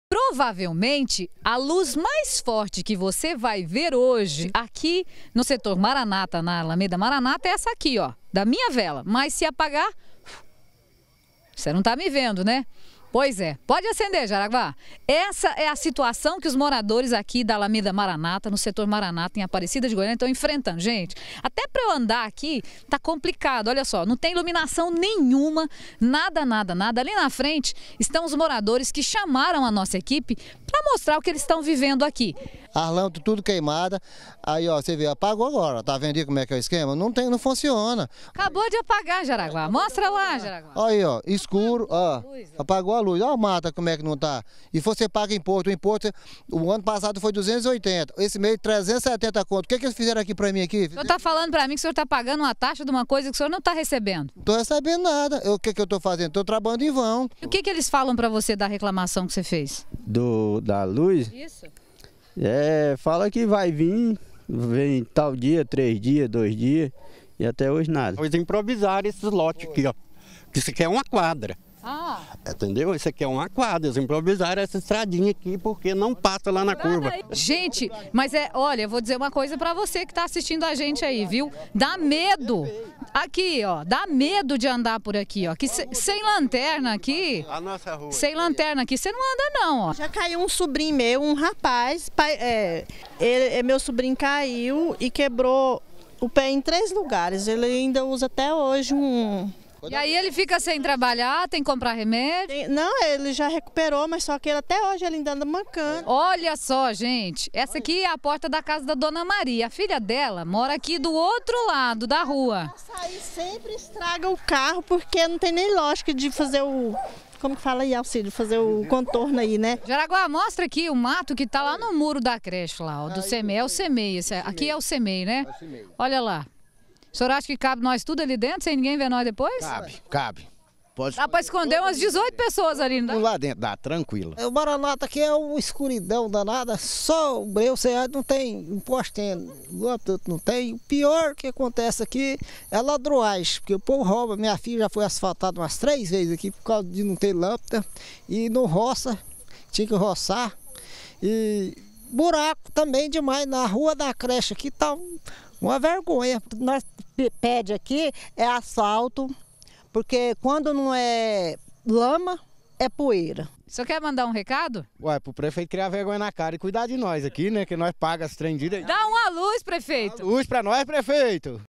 GA - Jd. Maranata: Sem luz, sem asfalto e mato alto - 27-02-2018
No Jardim Maranata, em Aparecida de Goiânia, os moradores tem uma lista de reclamação. Lá não tem iluminação, as ruas estão sem asfalto, buracos por toda parte,e mato alto é o que não falta. A única coisa que tem neste bairro é a insegurança e a revolta de quem paga os impostos e não tem o benefício.